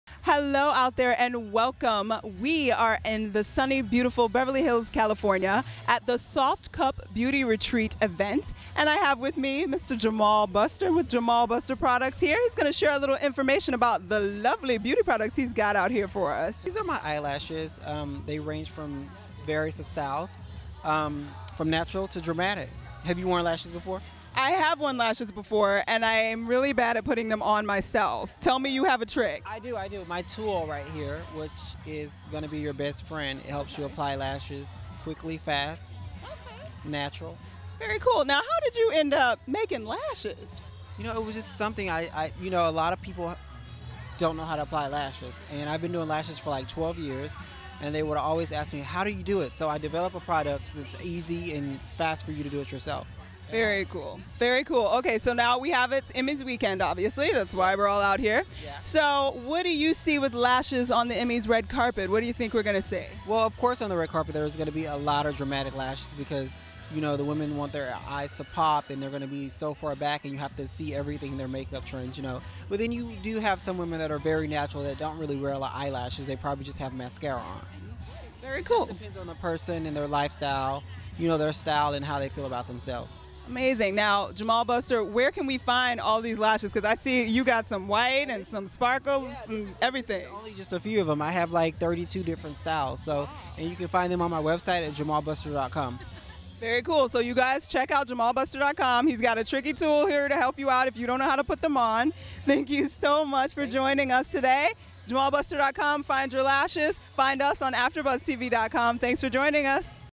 Emmy's Gift Suites | Softcup Beauty Retreat | Jamal Buster Eyelash Beauty Products | AfterBuzzTV
AfterBuzzTV Host Courtney Stewart attends the Softcup Beauty Retreat for the Emmy's and interviews Jamal Buster Beauty Products.

The Softcup Beauty Retreat kicked off the countdown to the Emmy's today at the lavish home of The Real Housewives of Beverly Hills star Adrienne Maloof (who coincidentally put her home on the market the same day for a whopping price of $26 million). Celebrity guests and tastemakers had the chance to relax, unwind and pamper themselves prior to the awards while enjoying delicious bites and libations created by Adrienne's private chef, indulging in various spa treatments, and sipping specialty cocktails by ZING Vodka, the Maloof family's brand new spirit venture.